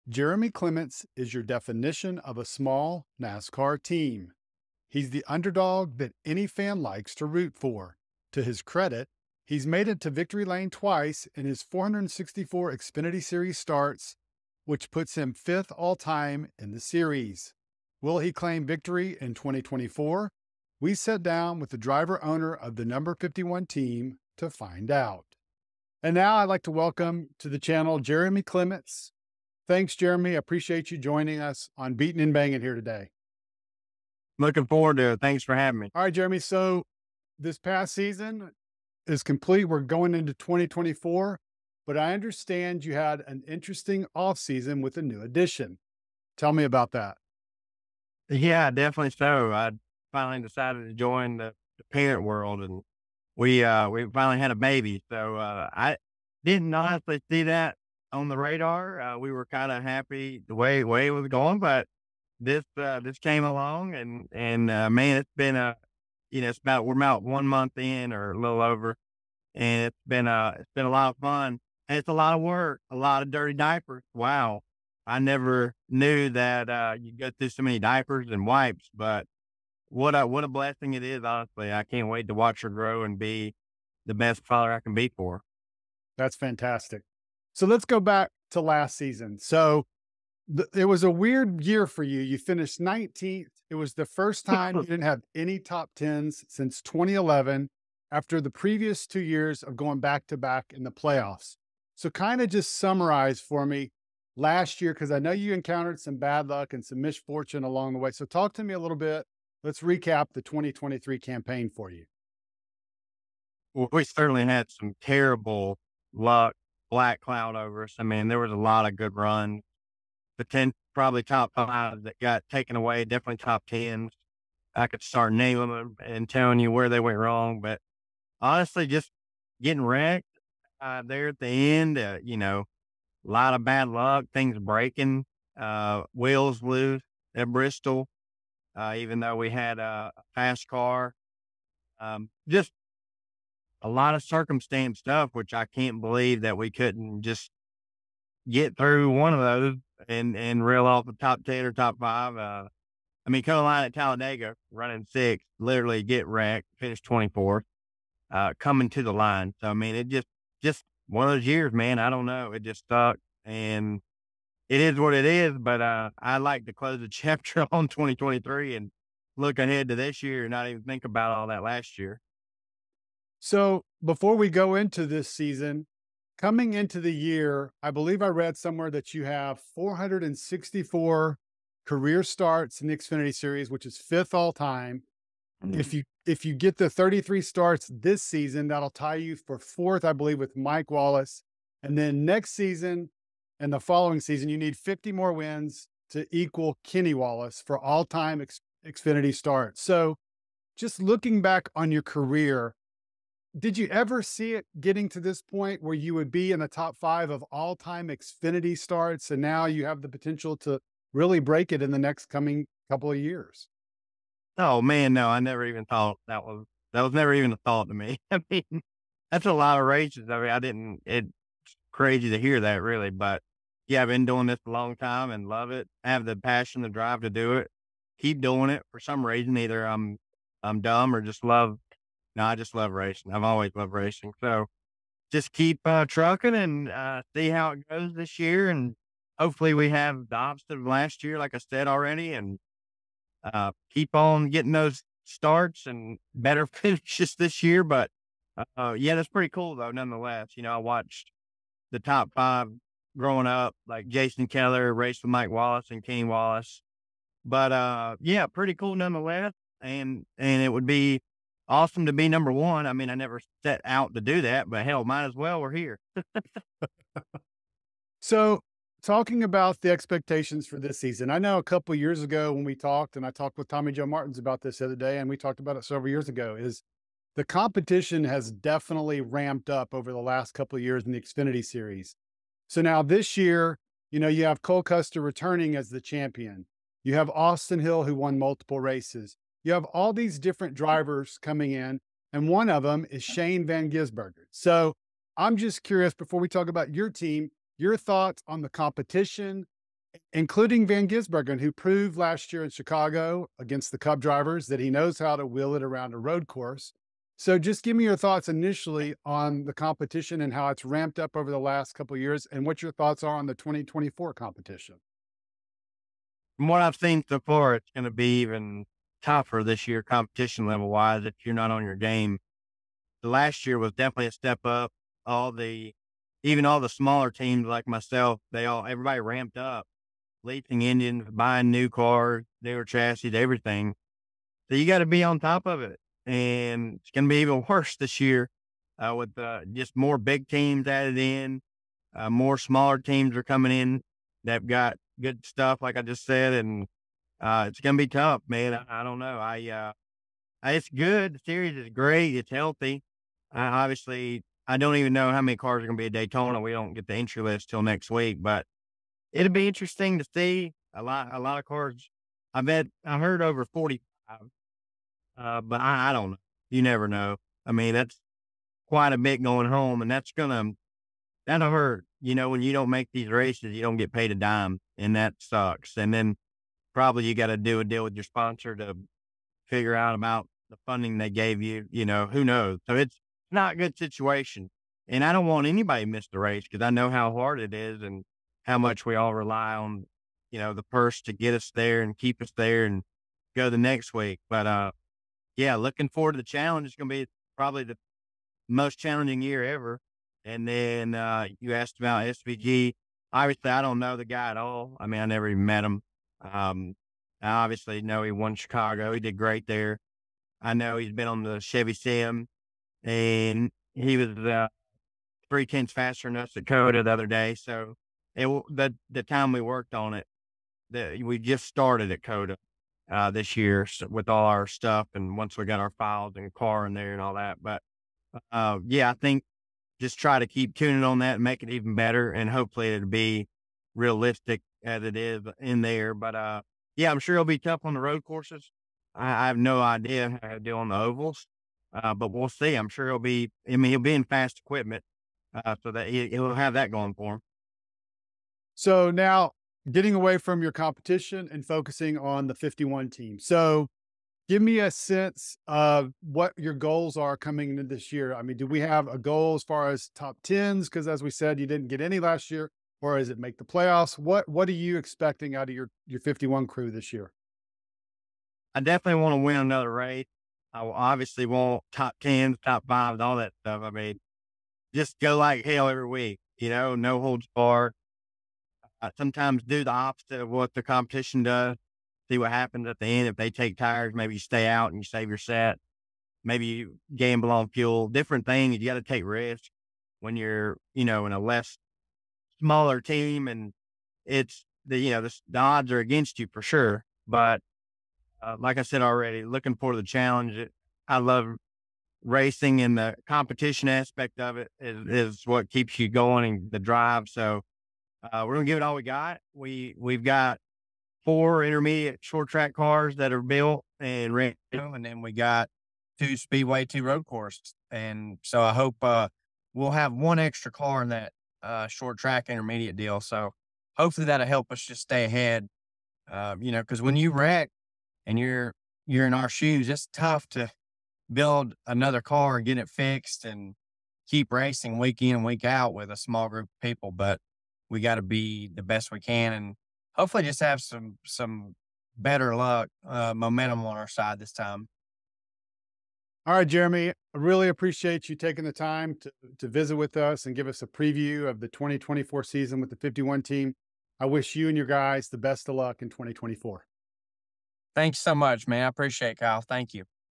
Jeremy Clements on 2024, Adding to Family, and Approaching 500 Xfinity Starts
Jeremy Clements is a NASCAR Xfinity Series veteran. How veteran? He's made 464 career starts in the series and is fifth all-time among all drivers. In the next couple of years, he'll pass 500 and possibly Kenny Wallace for most all-time in 2026.

The driver-owner talked about his career, what he expects out of the No. 51 team in 2024, and what life's been like this offseason with a new addition (and lots of dirty diapers) to the family.

Video Credits:
NASCAR
NBC
Fox Sports

Photo Credits:
Getty